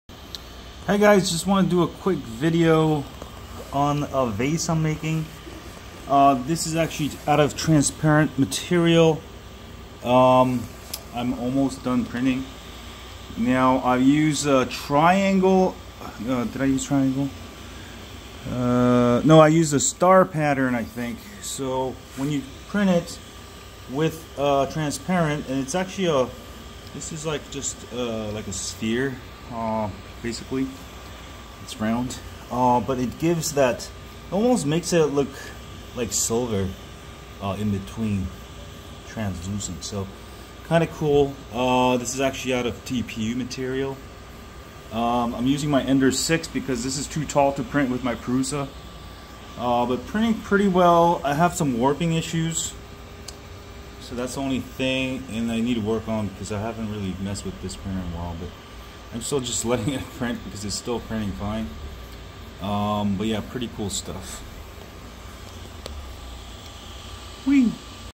Star pattern infill w/ transparent TPU filament gives you metallic look!
Star pattern infill w/ transparent TPU filament gives you metallic look!   1 layer perimeter and 30% star pattern infill.